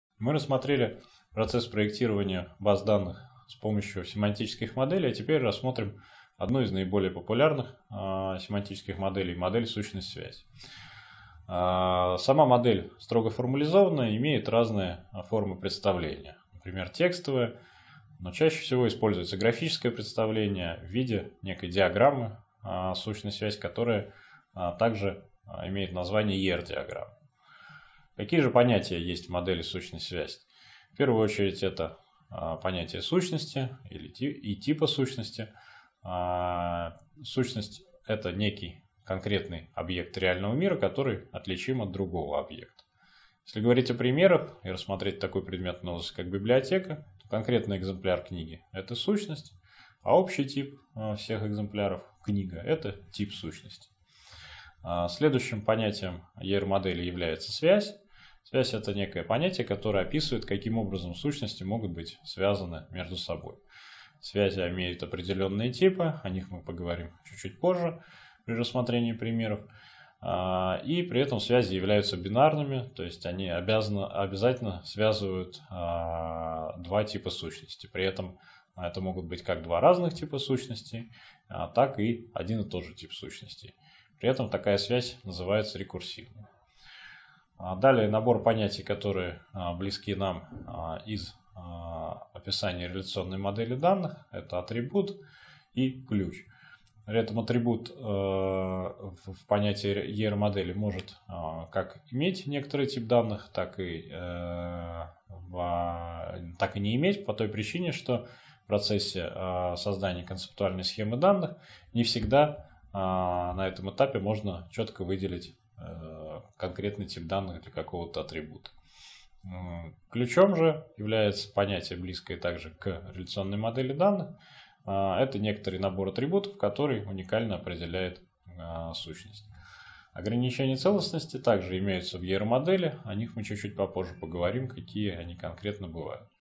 Введение в базы данных - 35 урок. Задача проектирования баз данных
Автор: Александр Мяснов, Иван Савин

Задание для закрепления:

Скажите, пожалуйста, к схеме данных какого типа преобразуются требования пользователей при проектировании БД с использованием семантических моделей?

* Концептуальная

* Физическая

* Логическая